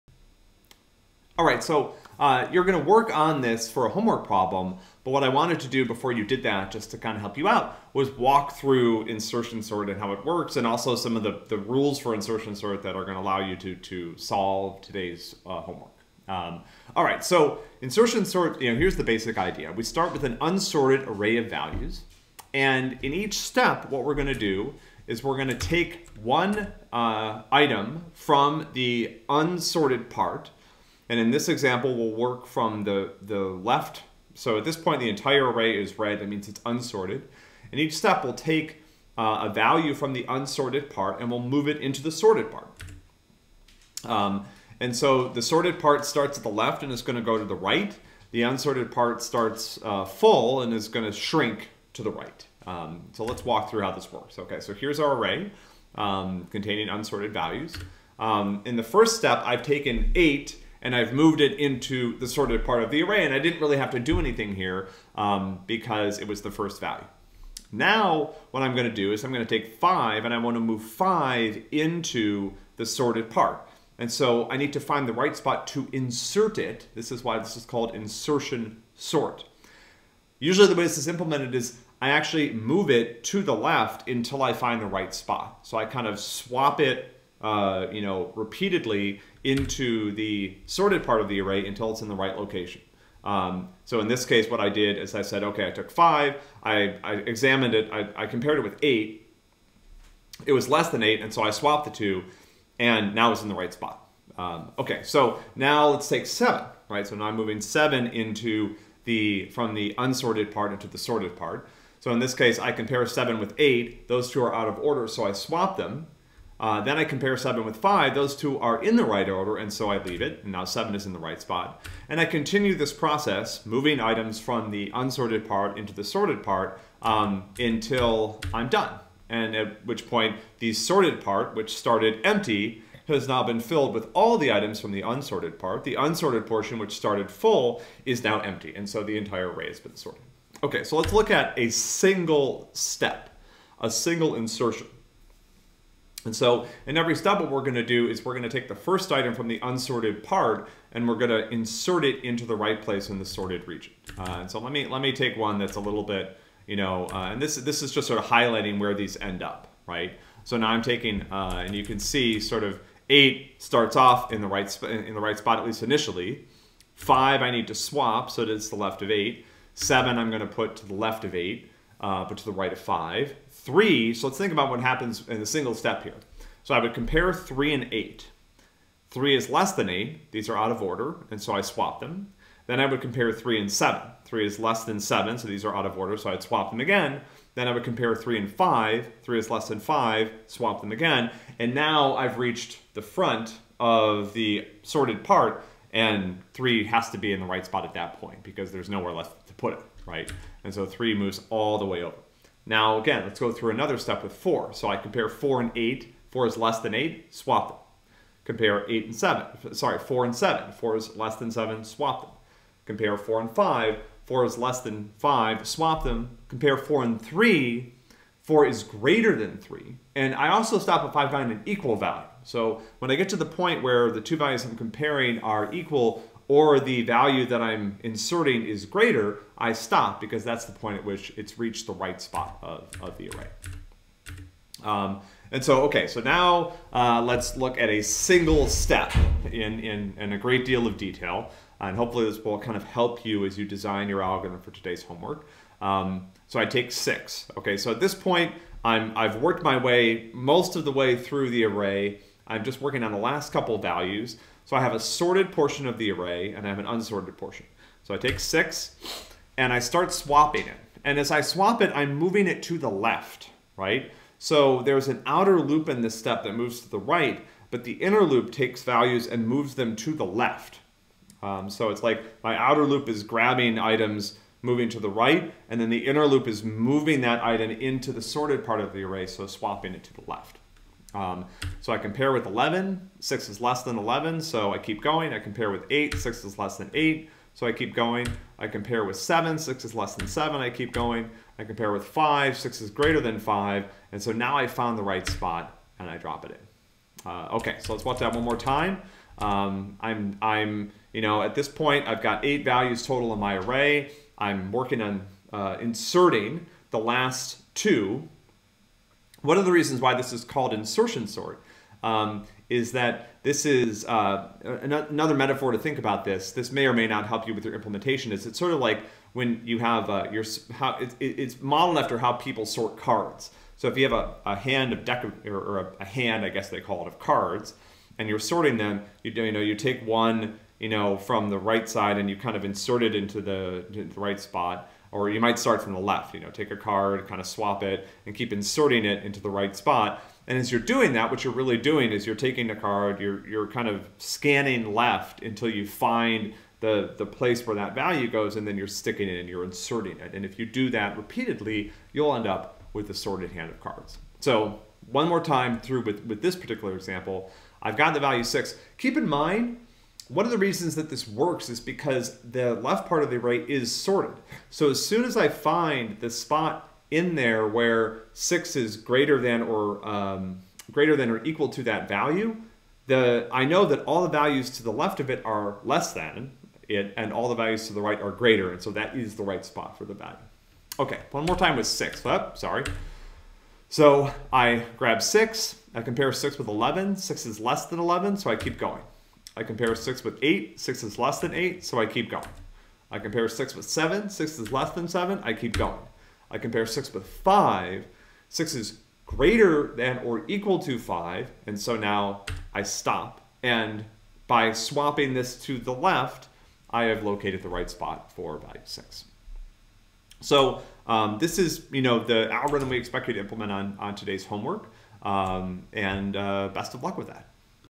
CS 125 Fall 2020 Lesson 56: Insertion Sort, Explained
A visual explanation of insertion sort.